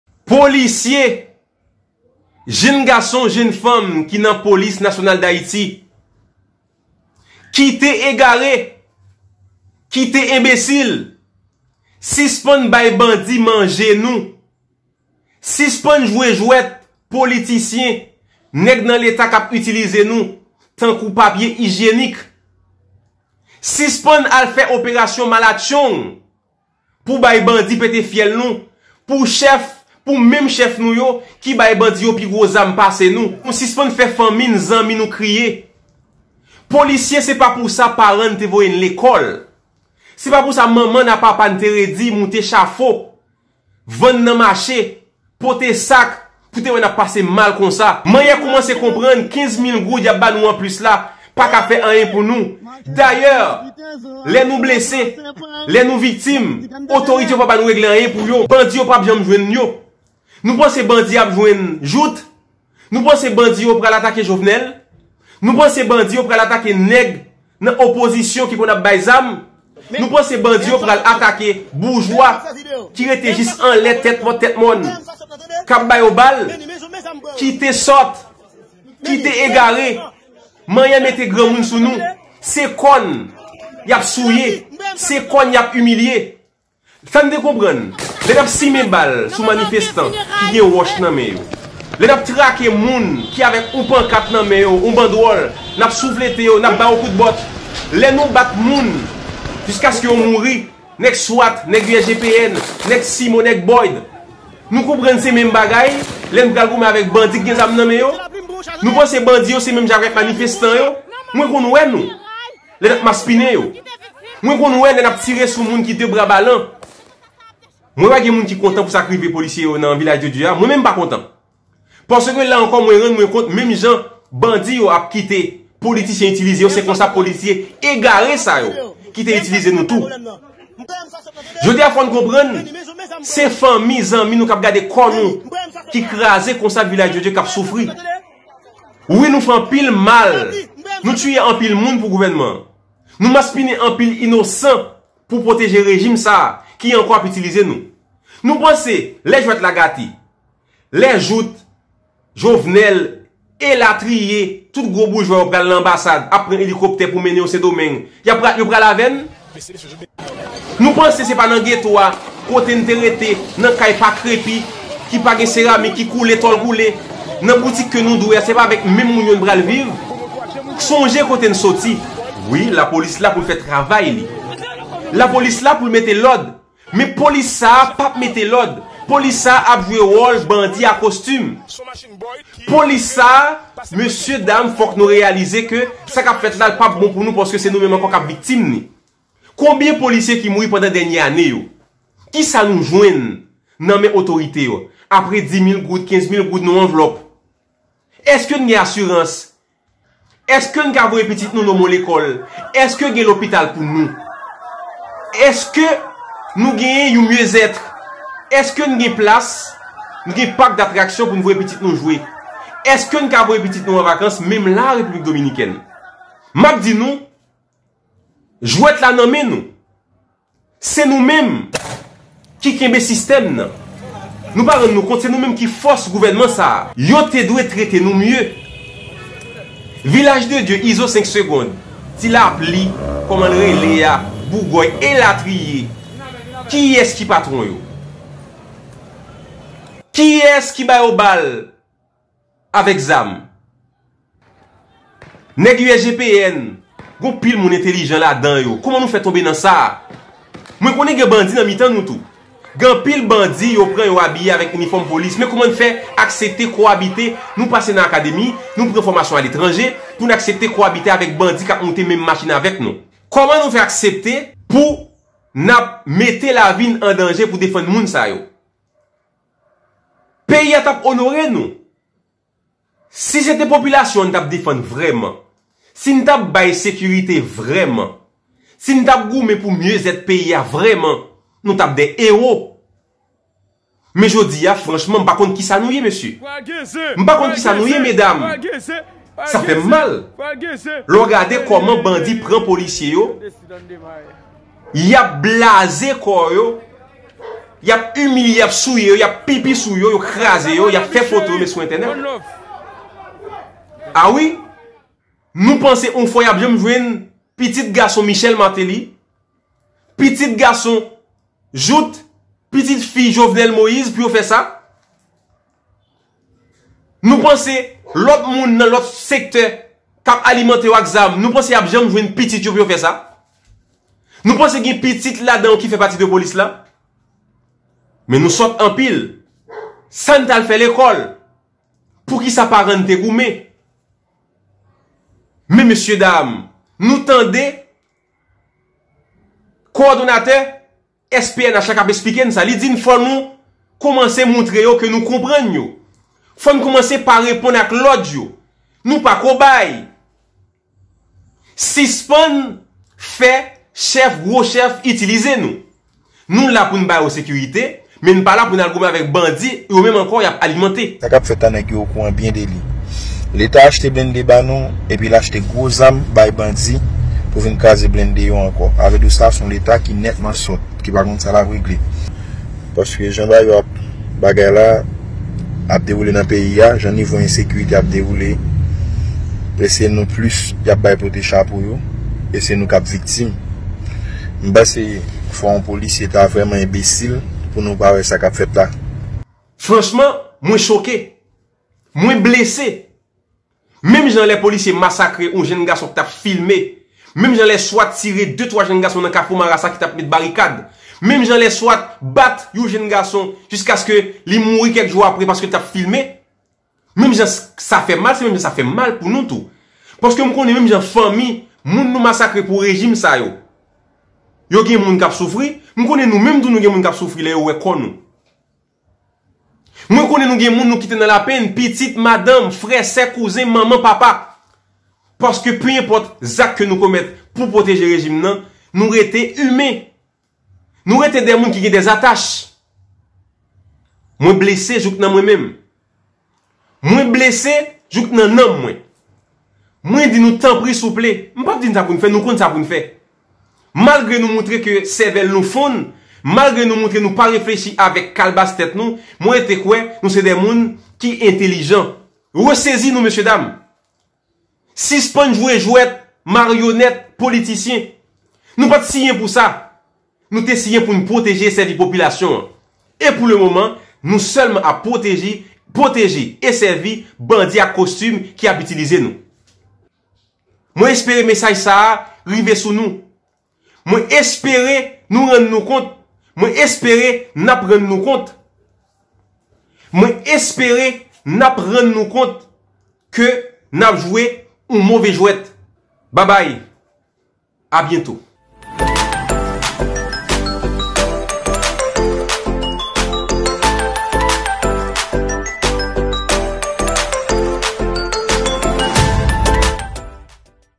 Polisye, kite enbesil😭😭 (depi 12 mas 2021 mwen te deja pale nou)
MESAJ IJAN pou polisye ki moun toujou anndan polis nasyonal la. Ede m fè l mache svp